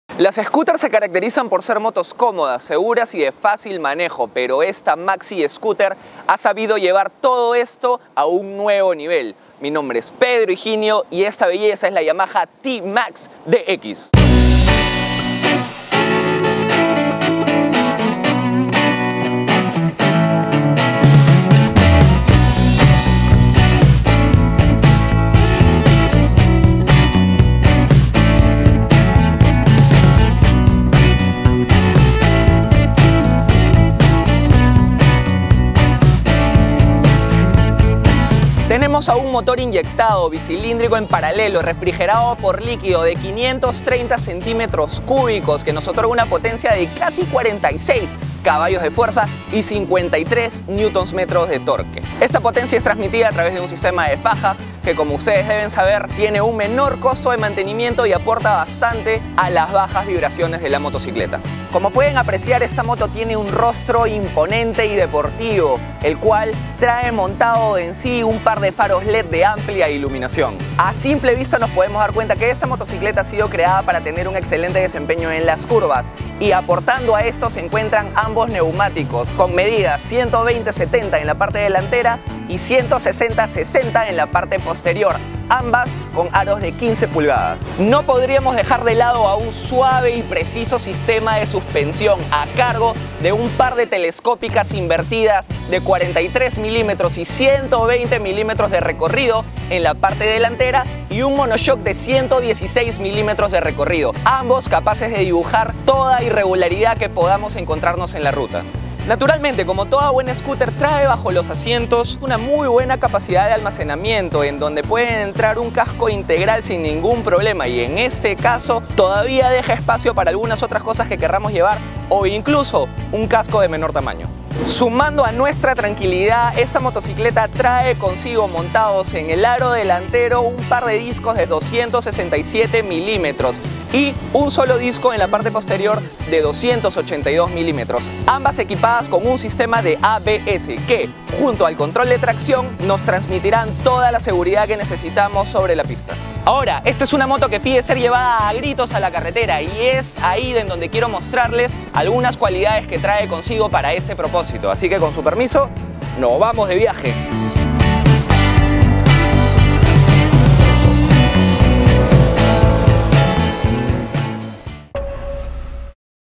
Yamaha - TMax DX (Review) PARTE 1 / Lima - Perú
La madre de todas las scooters de Yamaha y la mejor en su categoría. La TMax nos presenta este año su 6ta versión con algunas novedades y la misma excelencia de siempre.